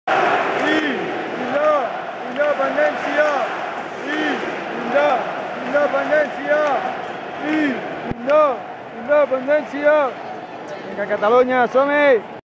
el Camp Nou canta "I-inde-independència !!" al minut 17 i 14 segons ( any 1714 ) contra el Granada
el Camp Nou .. pocs dies després de la manifestació del 11 de Setembre que va reunir 1,5 milions de catalans al carrer .. canta : "I-inde-independència !!" al minut 17 i 14 segons ( any 1714 ) contra el Granada el 22/09/2012
: el grup d'animació Almogàvers del Gol Sud van iniciar el càntic, onejant estelades, aconseguint que s'hi afegís la resta de la grada, i van reiterar el càntic diversos cops al llarg del partit, i quan es va sentir amb més força va esser al final del partit després d'aconseguir la victòria 2-0 "in extremis" ; més informació, fotos i vídeos a la web del seguidor i reporter més friki del Barça